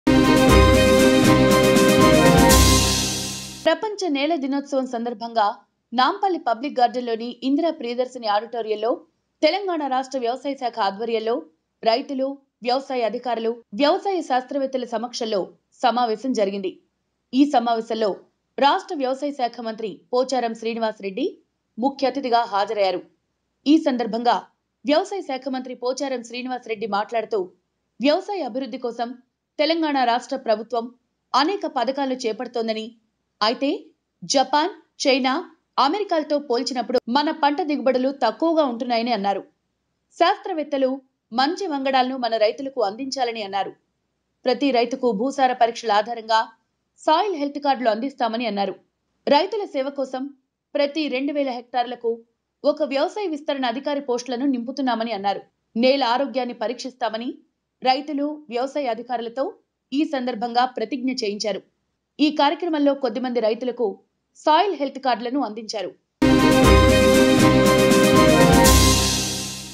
Pocharam Srinivas Reddy on agriculture development | Oneindia Telugu
ప్రపంచ నేల దినోత్సవం సందర్భంగా నాంపల్లి పబ్లిక్ గార్డెన్ లోని ఇందిరా ప్రియదర్శిని ఆడిటోరియంలో తెలంగాణ రాష్ట్ర వ్యవసాయ శాఖ ఆధ్వర్యంలో రైతులు, వ్యవసాయ అధికారులు, వ్యవసాయ శాస్త్రవేత్తల సమక్షంలో సమావేశం జరిగింది. ఈ సమావేశంలో రాష్ట్ర వ్యవసాయ శాఖ మంత్రి పోచారం శ్రీనివాస రెడ్డి ముఖ్యఅతిధిగా హాజరైయ్యారు. ఈ సందర్భంగా వ్యవసాయ శాఖ మంత్రి పోచారం శ్రీనివాస రెడ్డి మాట్లాడుతూ వ్యవసాయ అభివృద్ధి కోసం తెలంగాణ రాష్ట్ర ప్రభుత్వం అనేక పథకాలను చేపడుతున్నదని అయితే జపాన్, చైనా, అమెరికాలతో పోల్చినపుడు మన పంట దిగుబడులు తక్కువగా ఉంటున్నాయని అన్నారు.

Telangana agriculture minister Pocharam Srinivas reddy participated in a meeting.